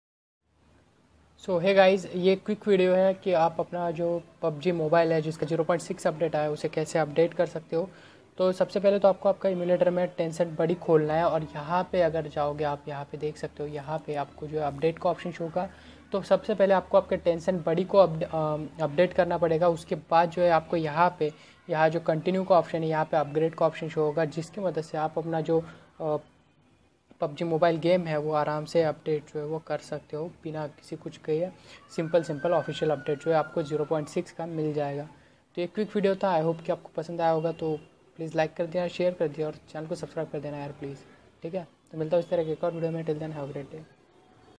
how to install pubg v0.8 on tencent gaming buddy
I will show you how to update pubg to 0.6.0 on tencent gaming buddy watch till the end and if you stuck ask in the comments